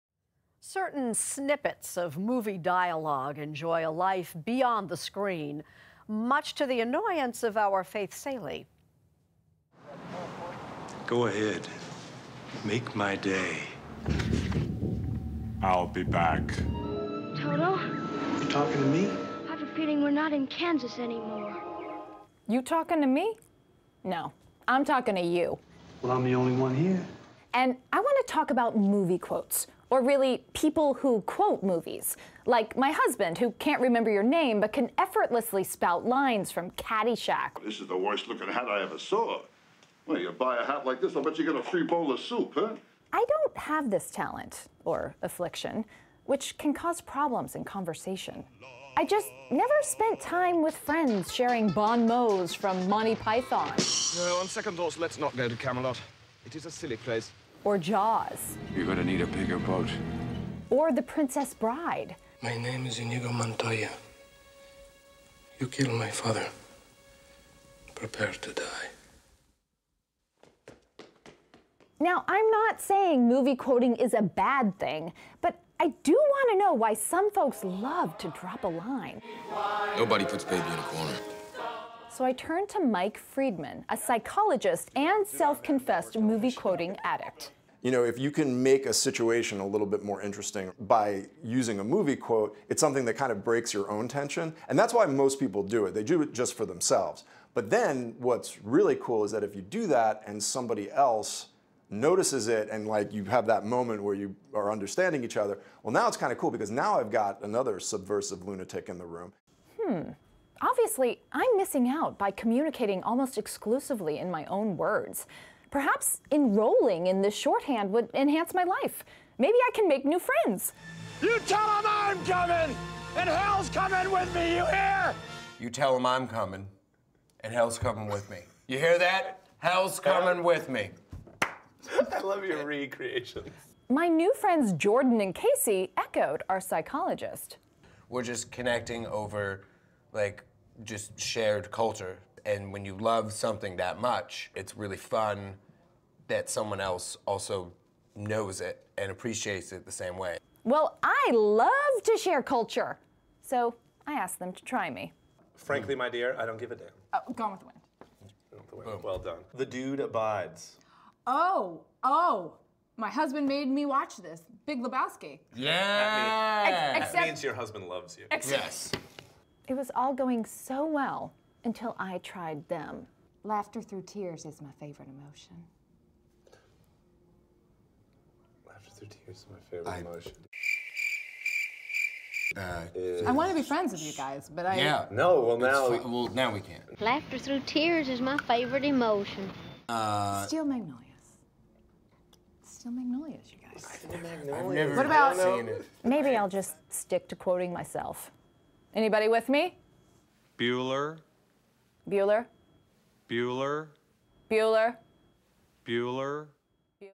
Why do people love to quote movies?
Faith Salie wants to know why some people eagerly quote from movies in everyday conversation - and why she can't.